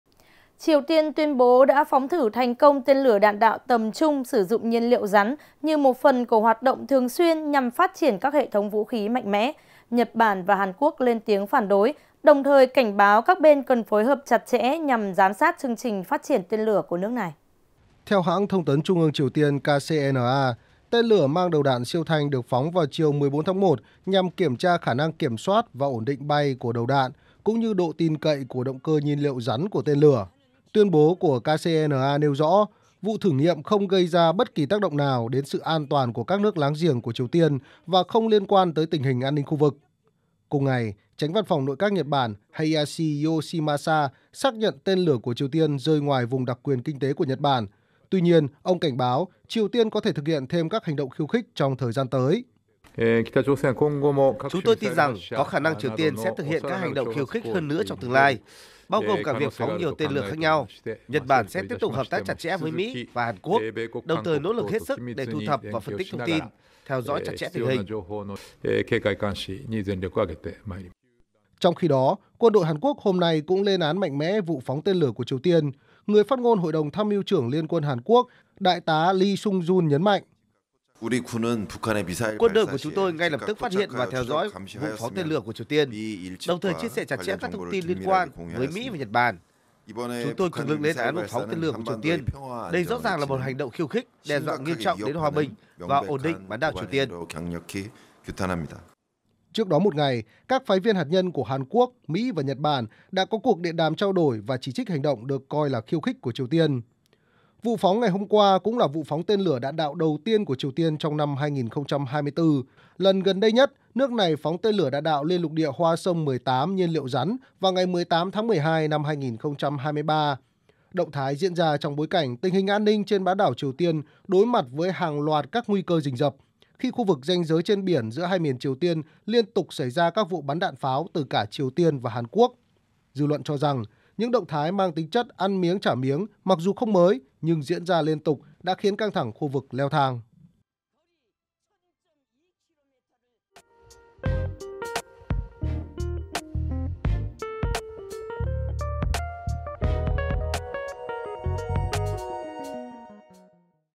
Nhật-Hàn cảnh báo khả năng Triều Tiên gia tăng tần suất các vụ phóng tên lửa | Báo Điện tử VOV
Báo Điện tử VOV | Triều Tiên ngày 15/1 tuyên bố đã phóng thử thành công tên lửa đạn đạo tầm trung sử dụng nhiên liệu rắn (IRBM), như một phần của hoạt động thường xuyên nhằm phát triển các hệ thống vũ khí mạnh mẽ. Nhật Bản và Hàn Quốc lên tiếng phản đối, đồng thời cảnh báo các bên cần phối hợp chặt chẽ nhằm giám sát chương trình phát triển tên lửa của nước này.

37 Bà Triệu, Hoàn Kiếm, Hà Nội, Việt Nam